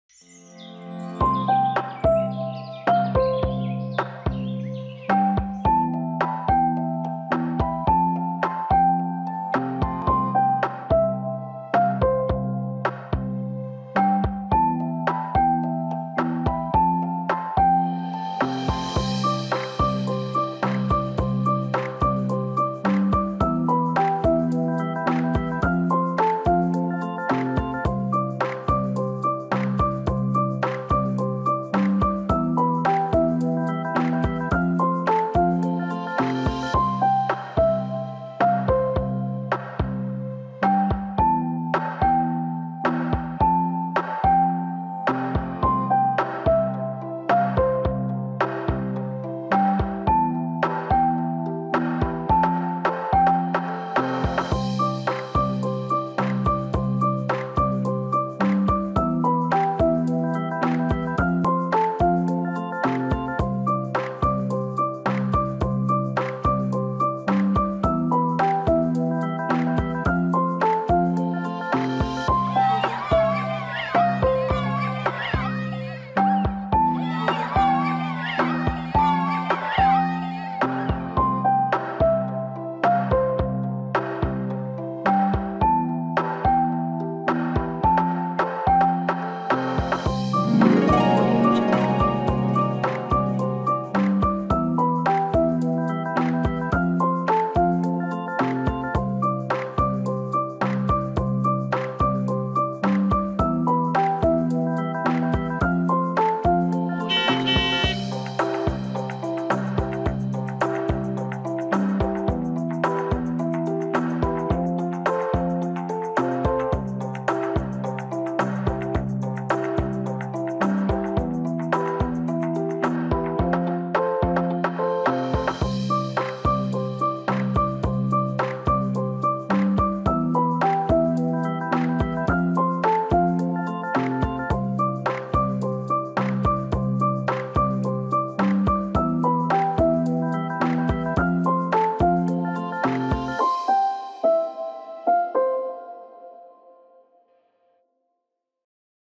Lemon Square Snack Time Stories | Cheesecake Story (w/o VO)
Teach your kid how to conquer big feelings like nervousness in A Cheesecake Story! 💛✨